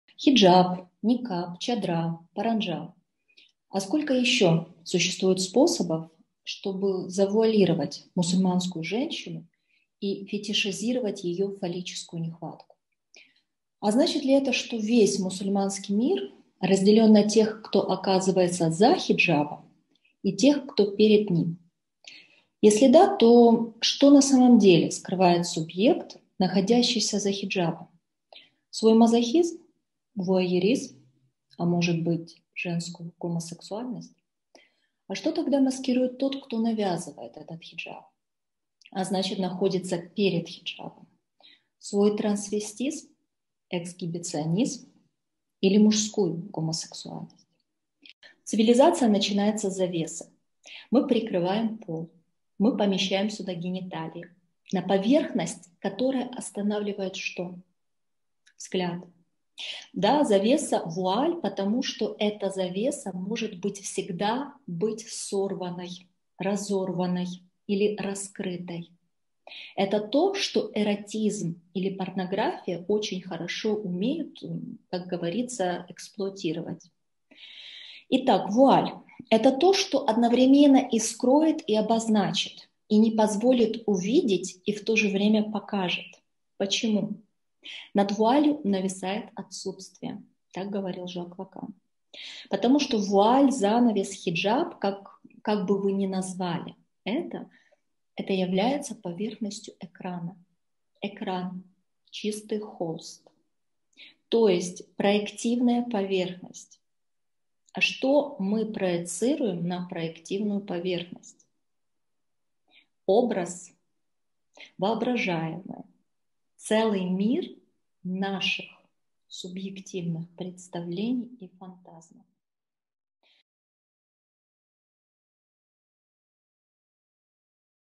Психоанализ о хиджабе/معنى الحجاب في التحليل النفسي
الحجاب والنقاب والشادور والبرقع وكم من الطرق الأخرى هناك لإخفاء امرأة مسلمة و تصنيم نقصها القضيبي؟